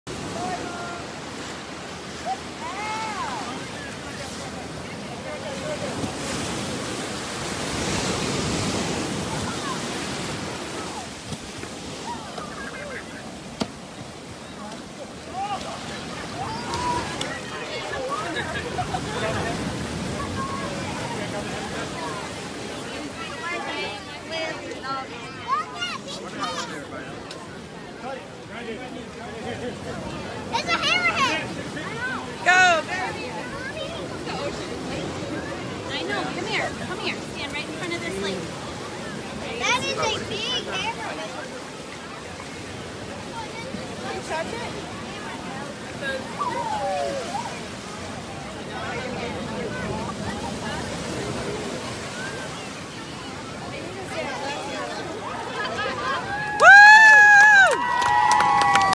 Huge Hammerhead Shark Caught and Released in Florida
An 8-foot hammerhead was caught and subsequently released back into the water on a beach in northern Florida recently, much to the excitement of onlookers.

'Never, in all my life living in Florida I have never seen one that big,' said Sharon Bunch Skipper, who recorded footage of the shark.